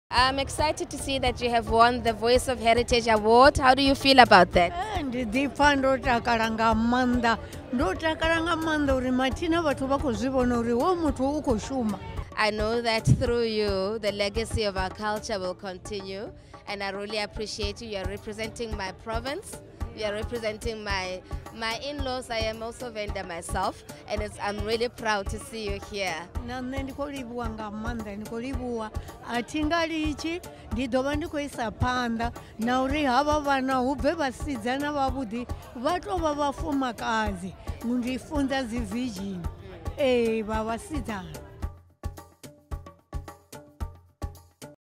National Heritage Awards 2018 | Winners' Interview: Naledzani
We were honoured to celebrate South Africa's greatest cultural ambassadors.